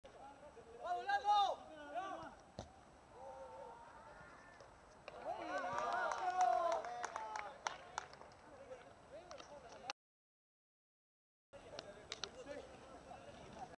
reyes, futbol calle 16, gol de pizarrón
Gol de arco a arco.